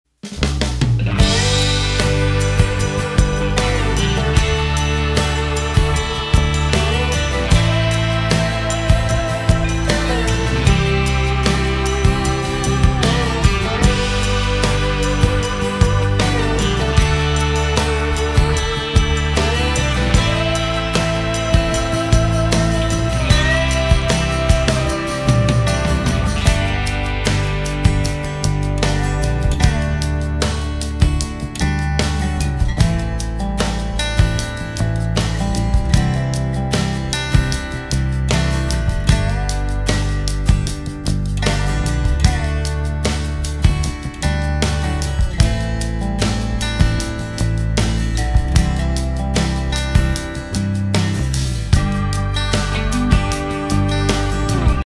Black Brooch Women Accessories Victorian Gothic Jewelry
Wiccan Pagan Jewelry Light and exquisite lace jewelry created in the technique of tatting, Ankars
This brooch is in a single copy, are splendid and original, nobody has something like this. This Elegant brooch is perfect to embellish a coat or a jacket, dress or suit. Beads sparkle and shimmer in the light.